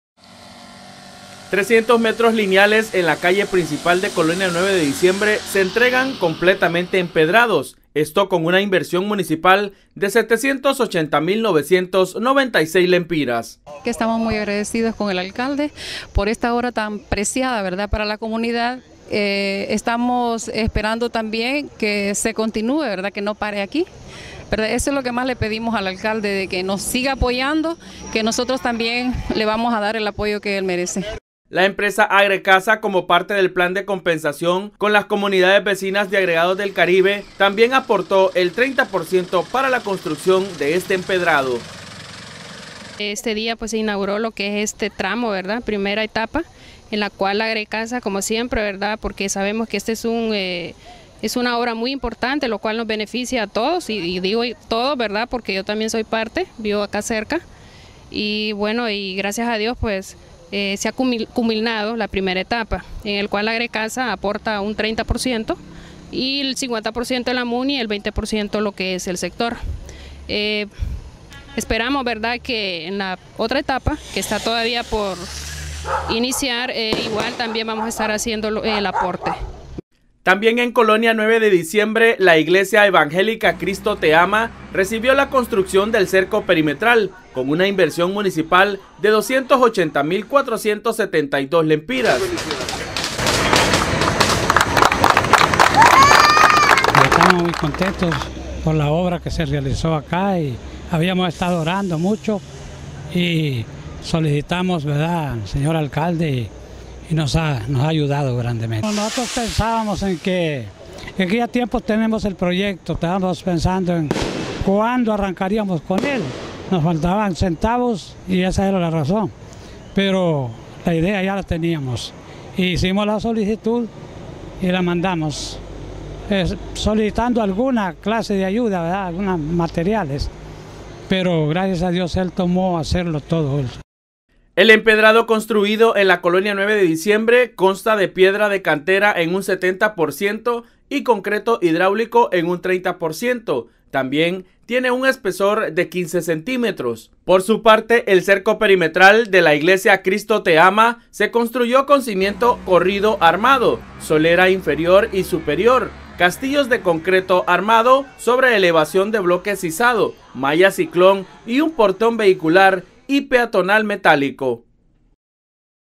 Municipalidad de Puerto Cortés, INAUGURACION EMPEDRADO Y CERCO PERIMETRAL EN COL 9 DE DIC.
Video producido por la Oficina de Relaciones de la Municipalidad de Puerto Cortés